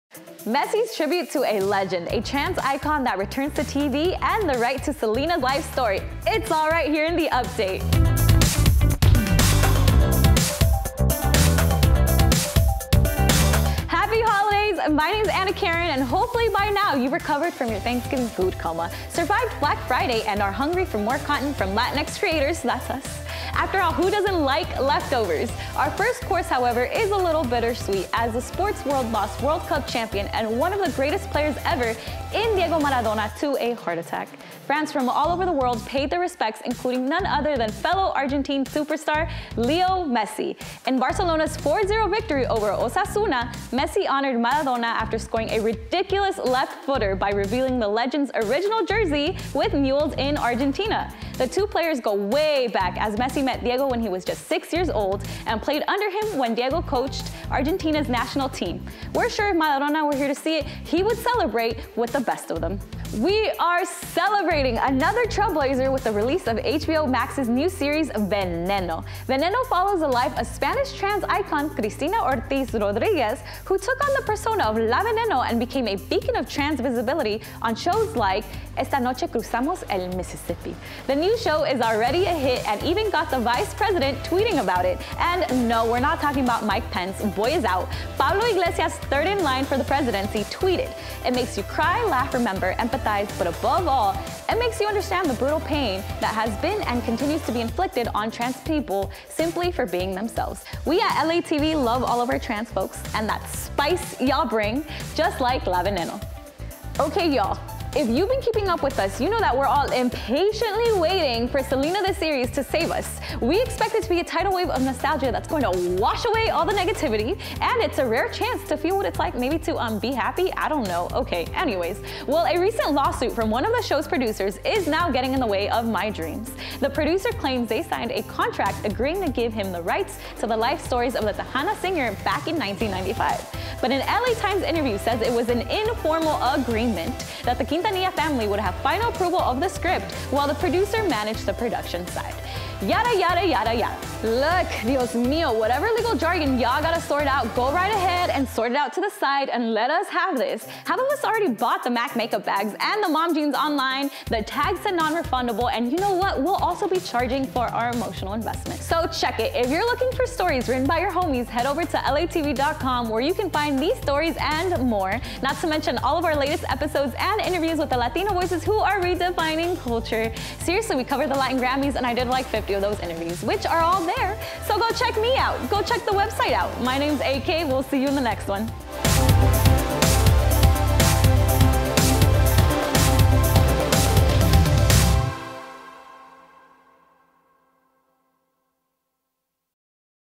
Messi Honors Maradona, A Trans Icon Returns, and a Selena Lawsuit | LATV Update
- Leo Messi honors the late Diego Maradona after a 4-0 win against Osasuna.
- Spanish Trans icon La Veneno returns to television in HBO Max's Veneno.
- The rights to Selena Quintanilla's story is in dispute over a recent lawsuit filed by the series producer.

It can all be found in the latest update!